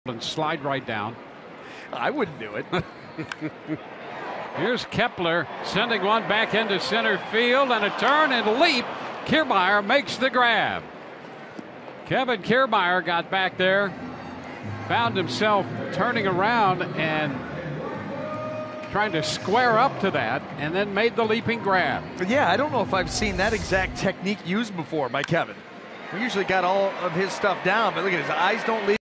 TB@MIN: Kiermaier smoothly makes jumping catch
Kevin Kiermaier shows off his Gold Glove resume by easily hauling in a line drive from Max Kepler in the bottom of the 4th inning